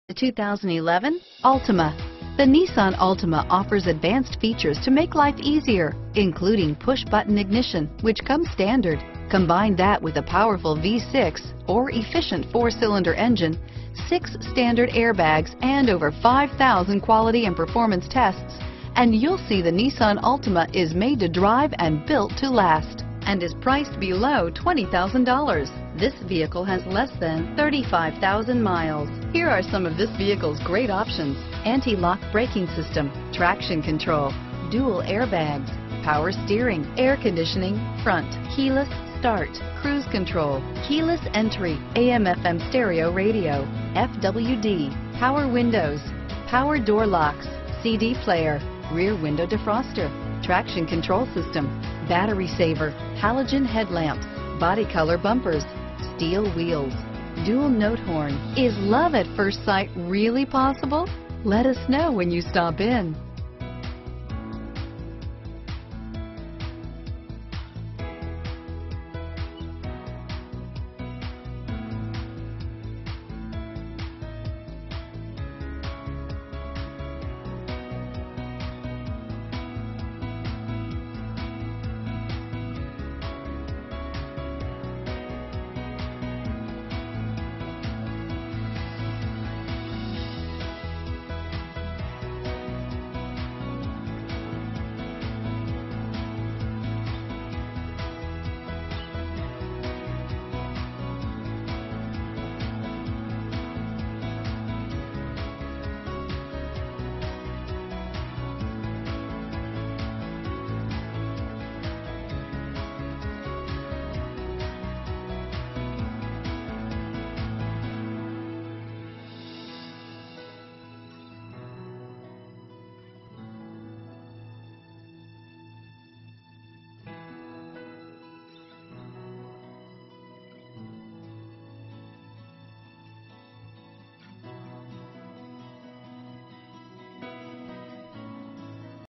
2011 Nissan Altima - 2.5 S Sedan 4D Phoenix AZ 520220
2011 Nissan Altima  - Stock#: 520220

CVT with Xtronic, ABS brakes, Electronic Stability Control, Low tire pressure warning, Remote keyless entry, and Traction control. Only one owner! Don't miss this double-bargain of saving at the dealership AND at the pump! Your time is almost up on this fantastic-looking 2011 Nissan Altima. Awarded Consumer Guide's rating as a 2011 Recommended Midsize Car. In 2007, Nissan redesigned the Altima, and took a top-5 selling vehicle, and made it significantly better, offering more luxury, extra power, and refined styling. This 2011 Altima 2.5 S marked a freshening of that design the previous year, with a restyled front fascia, standard stability control, and numerous other minor improvements.